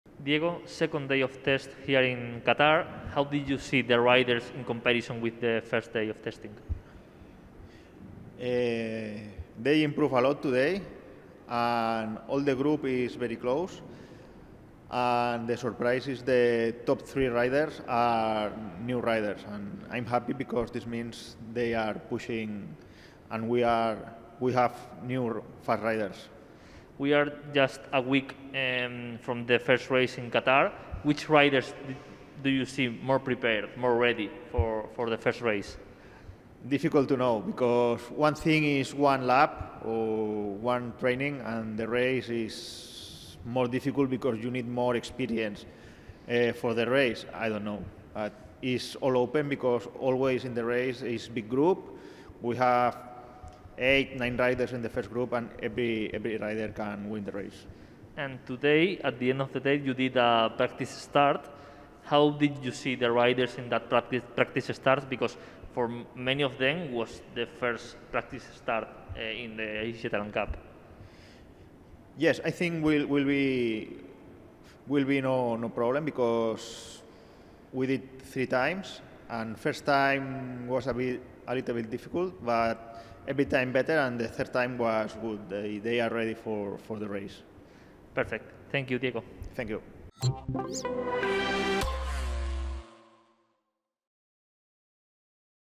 Diego Lozano interview Day 3 | 2022 Pre-Season Test
The test is completed, and here's what the rider's coach Diego Lozano had to say after some productive days 🎙️